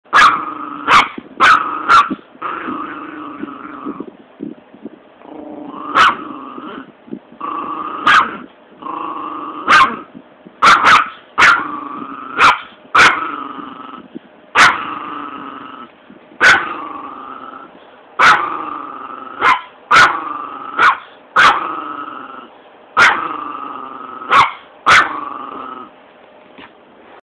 Pint sized bully
Jack likes to grab some kibble and bring it up to the bed to eat.  Pecas and cookie were up on the bed minding their own business when Jack started doing this